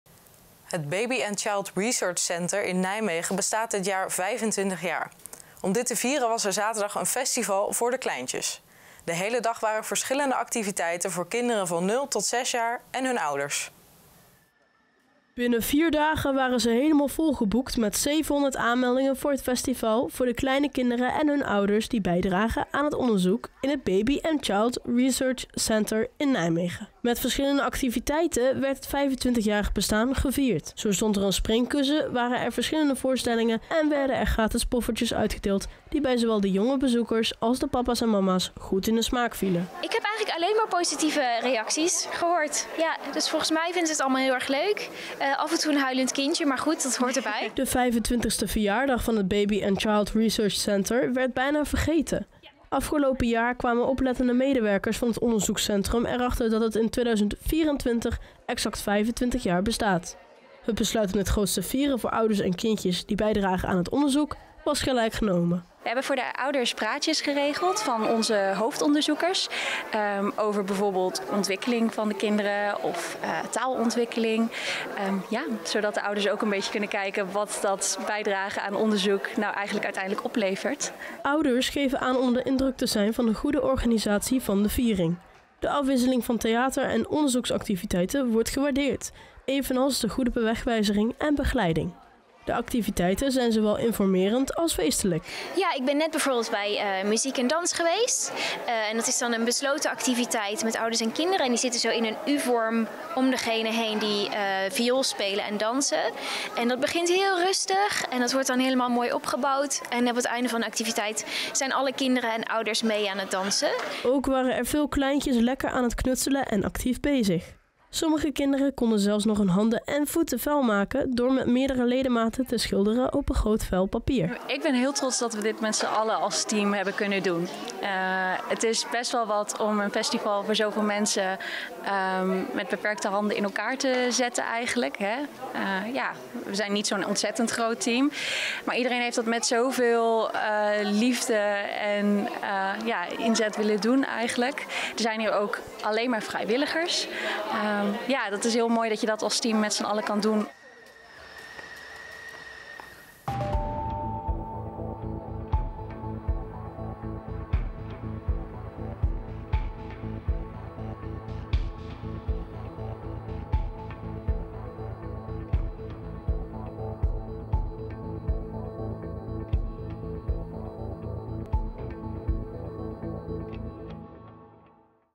25 Jaar Baby & Child Research Center Nijmegen || RN7 REGIONIEUWS
Het Baby & Child Research Center in Nijmegen bestaat dit jaar 25 jaar. Om dit te vieren was er op 1 juni een festival voor de kleintjes. De gehele dag waren er verschillende activiteiten voor kinderen van 0 tot 6 jaar en hun ouders.